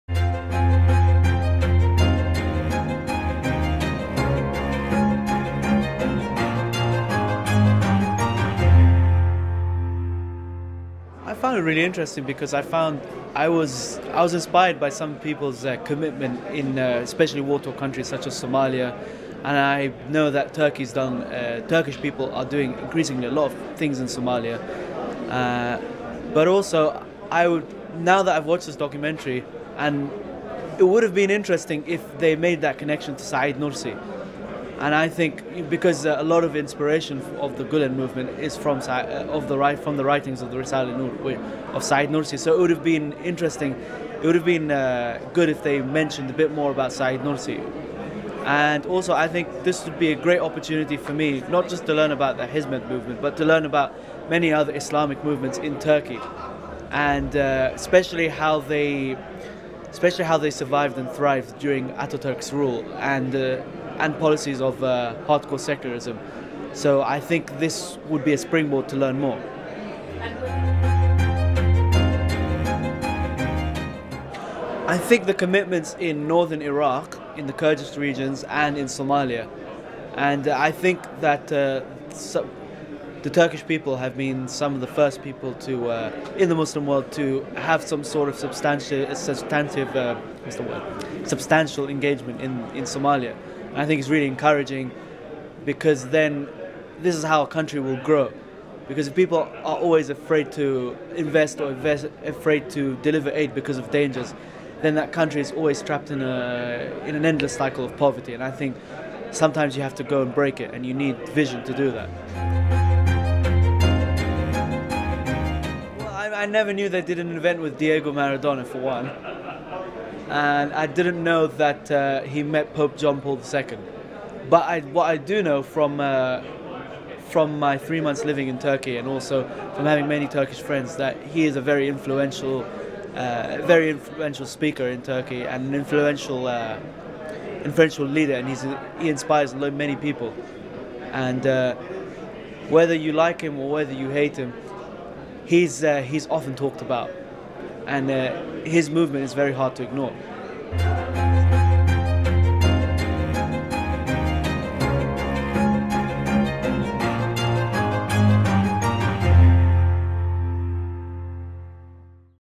Interview with Salahaddin Mazhary at London Premiere of Love is a Verb
Love is a Verb is a documentary by Terry Spencer Hesser exploring the ordinary lives and stories of people in Hizmet, a social movement inspired by the Islamic scholar and teacher, Fethullah Gülen, and geared towards serving all people regardless of their faith and religion through dialogue, education and relief work. The documentary was narrated by Hollywood actress Ashley Judd and produced by Global Vision Productions.

The London premiere of Love is a Verb was organised by the Centre for Hizmet Studies and Dialogue Society. It took place at the Leicester Square Empire Cinema on 4th November 2014.

New interviews with members of the audience will be released in the coming days and weeks.